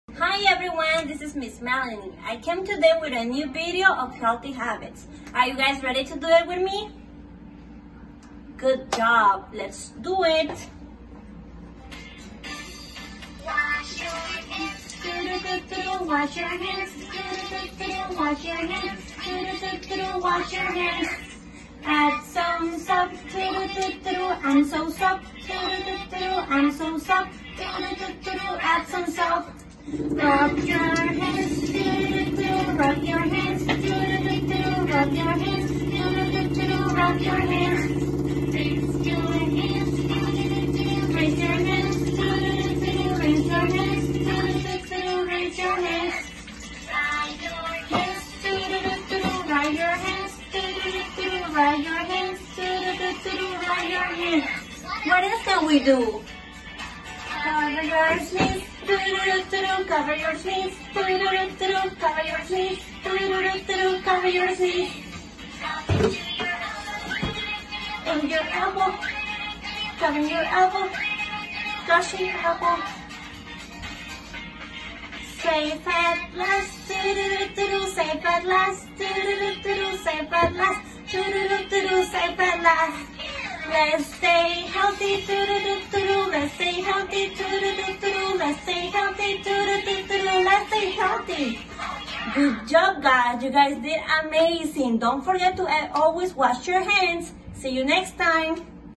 Healthy Habits with Ms. Melanie
Due to the recent virus that is going around, we are being advised by professionals to wash our hands frequently. For that, we have Ms. Melanie who makes washing our hands FUN with a new song, no one has heard.